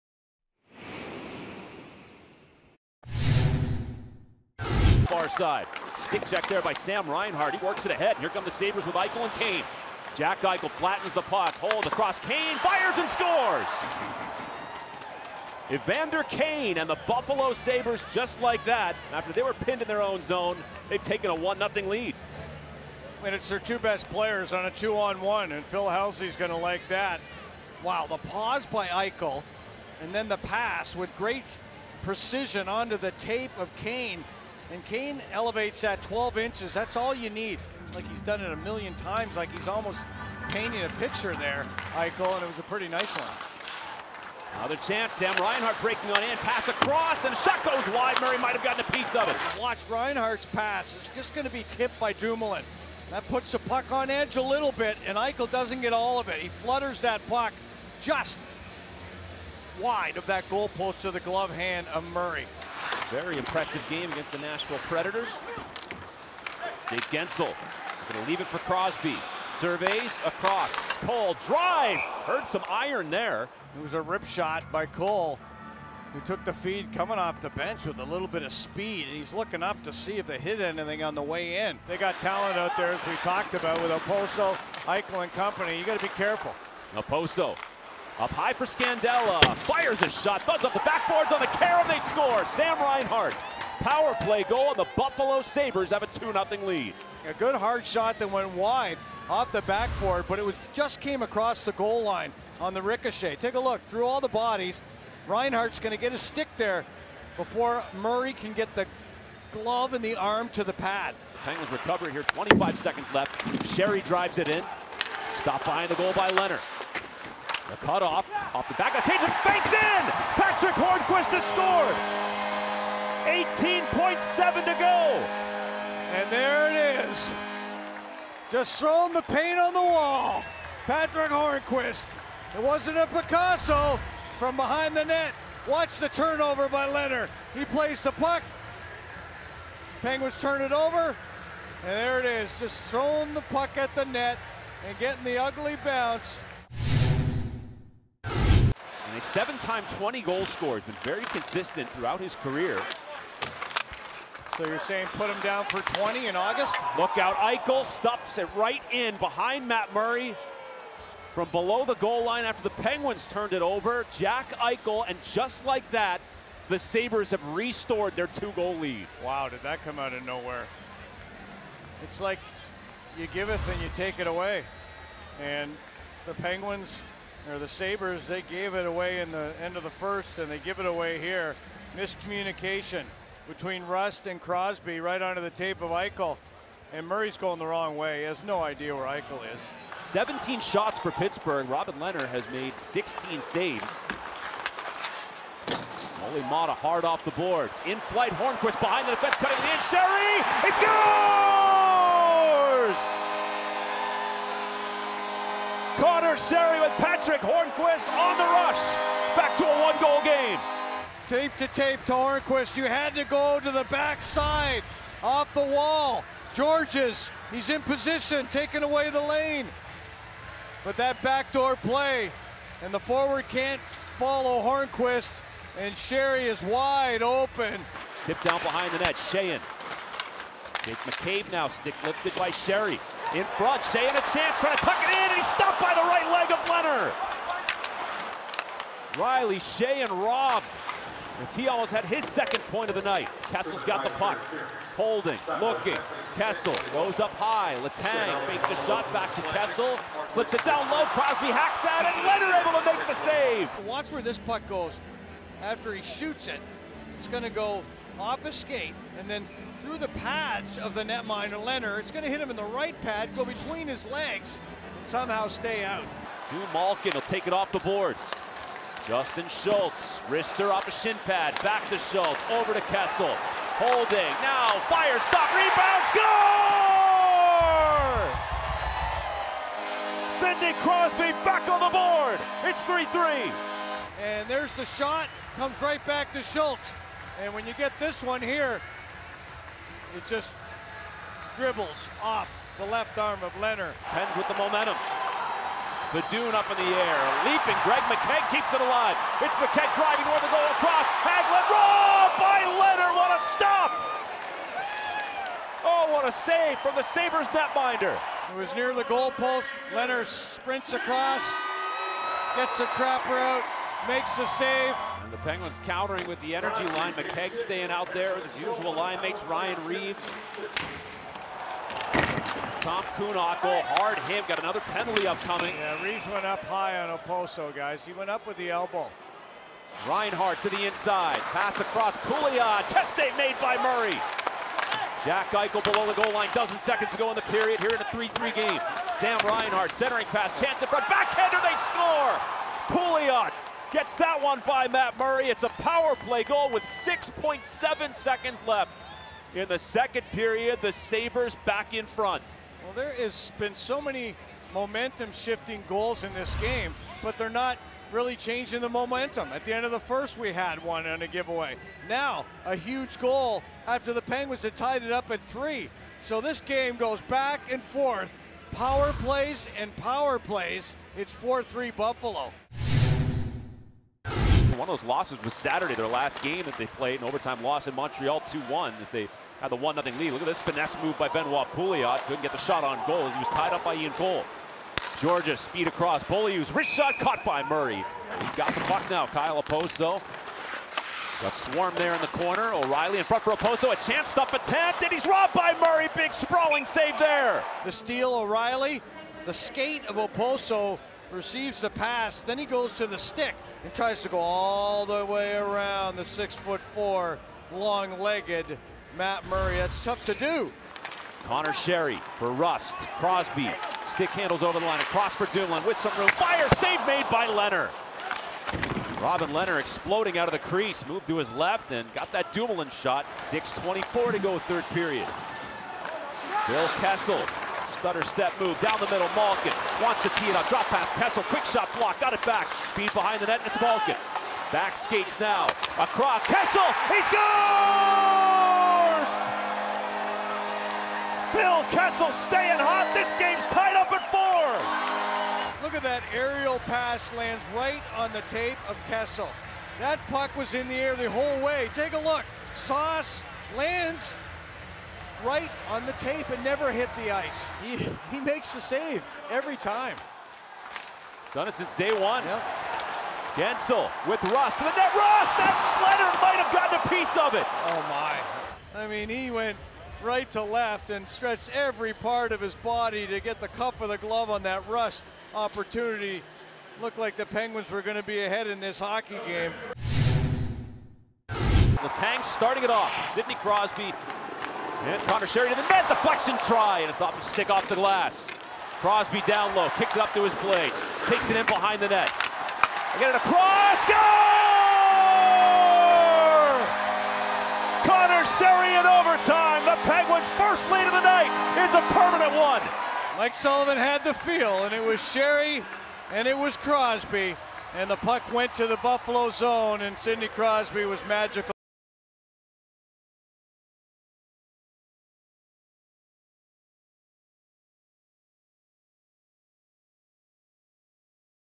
Питтсбург – Баффало (15.11.17) Обзор матча...
Питтсбург-
19 Хернквист (Шейэн)
24 Шири (Мяяття, Хернквист)
37 Кросби (Шульц, Кессел)
53 Кессел (Коул, Малкин)
60 Шири (Летанг, Кросби)
Баффало-
3 Кэйн (Райнхарт, Айкел)
16 Райнхарт (Окпосо, Сканделла)
20 Айкел
39 Пулио (Райнхарт)